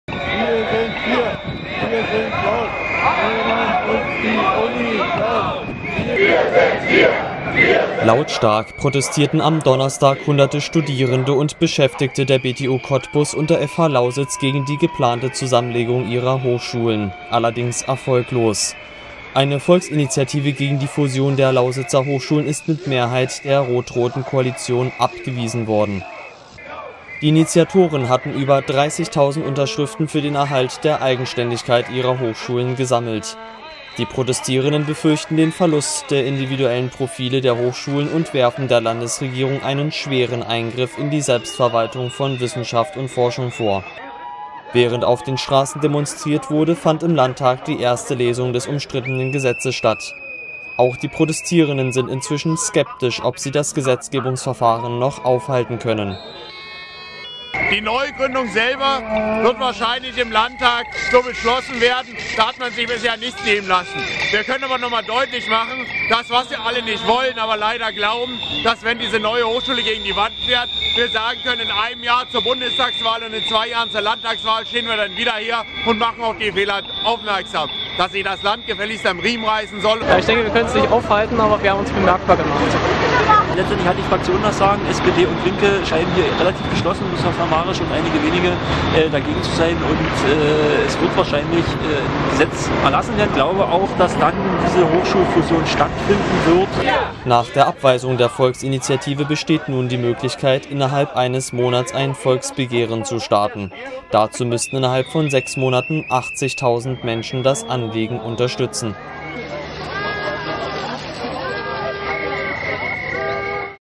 Protest gegen Hochschulen-Fusion erfolglos: Landtag weist Volksinitiative ab
Lautstark protestierten am Donnerstag hunderte Studierende und Beschäftige der BTU Cottbus und der FH Lausitz gegen die geplante Zusammenlegung ihrer Hochschulen -- allerdings erfolglos: Eine Volkinitiative gegen die Fusion der Lausitzer Hochschulen ist mit der Mehrheit der rot-roten Koalition abgewiesen worden. Die Initiatoren hatten über 30.000 Unterschriften für den Erhalt der Eigenständigkeit ihrer Hochschulen gesammelt. Die Protestierenden befürchten den Verlust der individuellen Profile der Hochschulen und werfen der Landesregierung einen schweren Eingriff in die Selbstverwaltung von Wissenschaft und Forschung vor.
Während auf den Straßen demonstriert wurde, fand im Landtag die erste Lesung des umstrittenen Gesetzes statt. Auch die Protestierenden sind inzwischen skeptisch, ob sie das Gesetzgebungsverfahren noch aufhalten können.
Nach der Abweisung  der Volksinitiative besteht nun die Möglichkeit, innerhalb eines Monats ein Volksbegehren zu starten. Dazu müssten innerhalb von sechs Monaten 80.000 Menschen das Anliegen unterstützen.
Mehr Informationen auf speakup.to